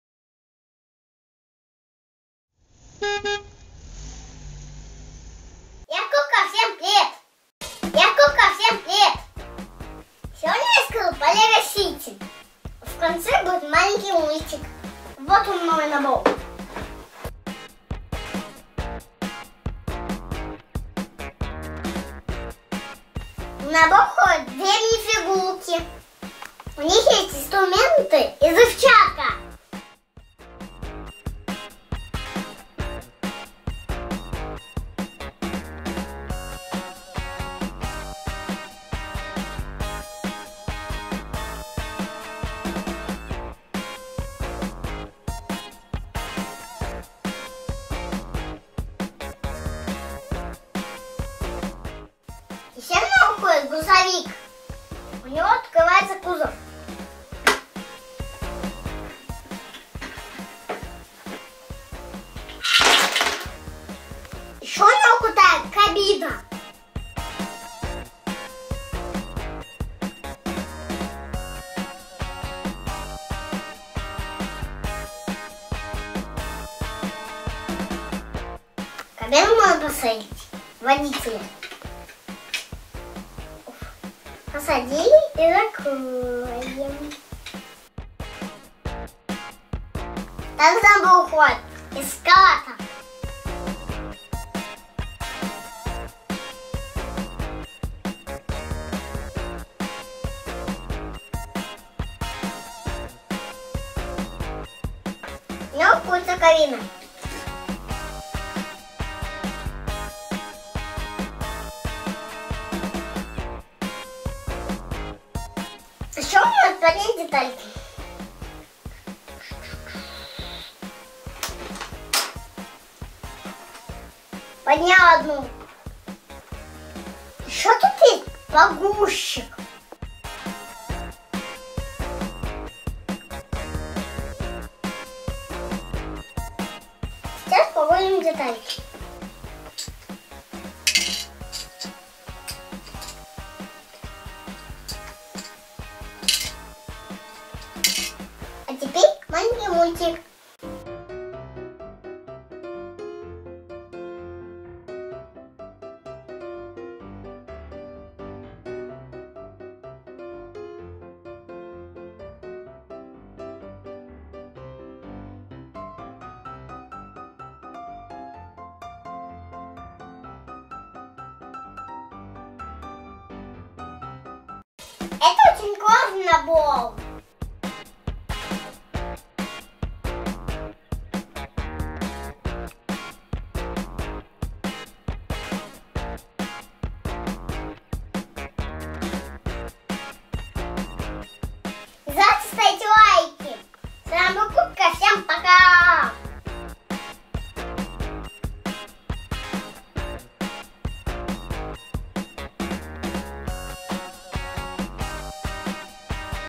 Лего Сити 60075 - Экскаватор и Грузовик - на русском. Лего Сити 2015. Лего Мультики. Кока туб
Выпуск 161: Лего Сити 60075 - Экскаватор и Грузовик - на русском. Лего Сити 2015. Лего Мультики. Кока туб
Привет, новый выпуск про Лего Сити, набор 2015 года - 60075. В конце малюсенький мультик)
2:30 - Лего Мультик

Пока-пока)